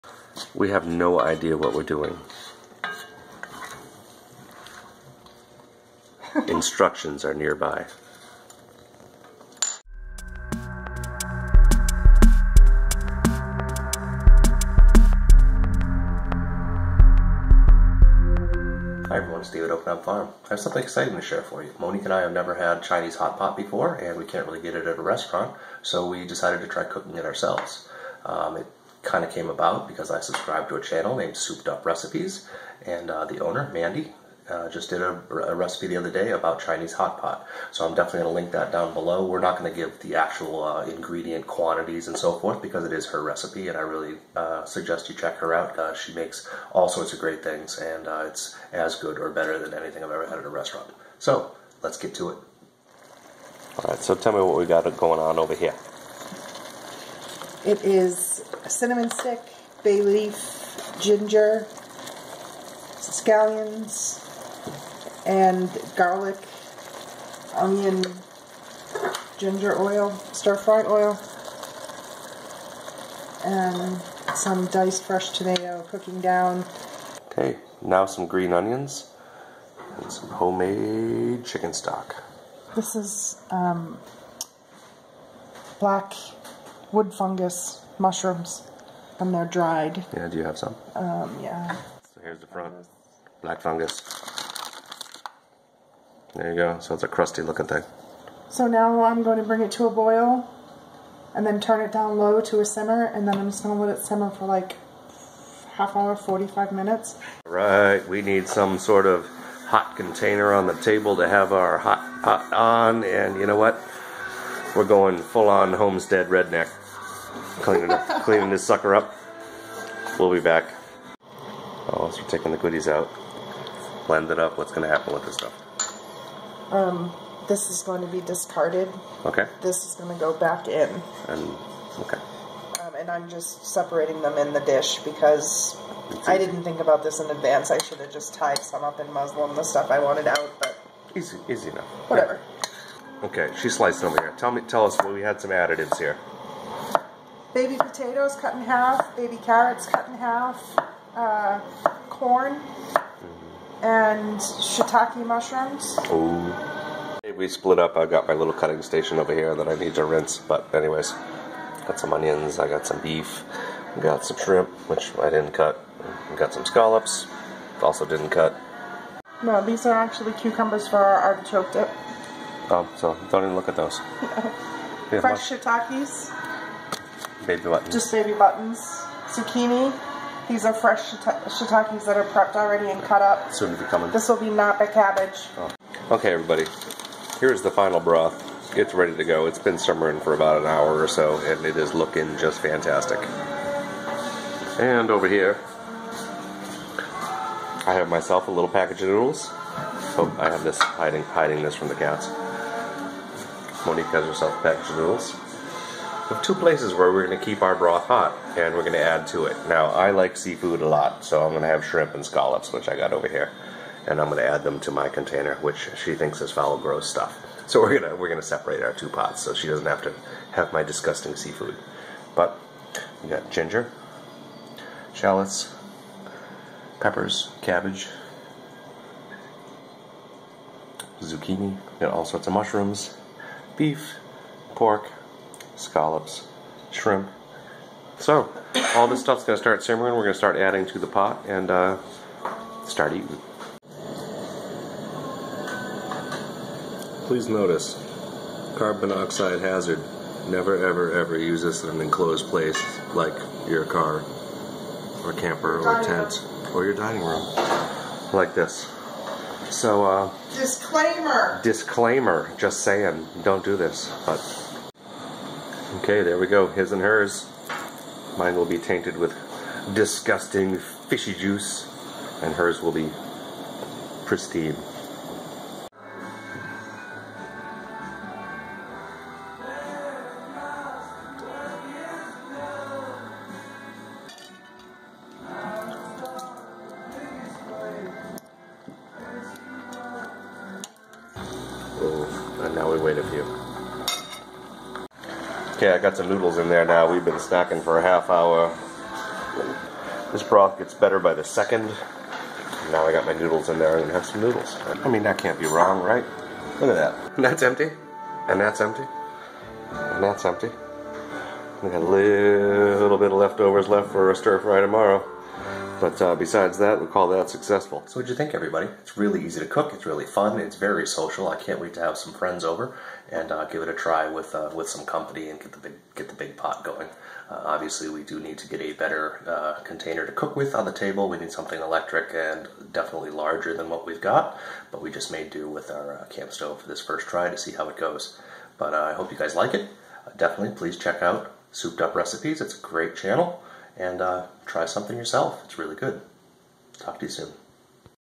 Our First Time Cooking Chinese Hot Pot
We decided to try cooking Chinese hot pot after seeing a video on "Souped Up Recipes".  It was easy, fun and delicious!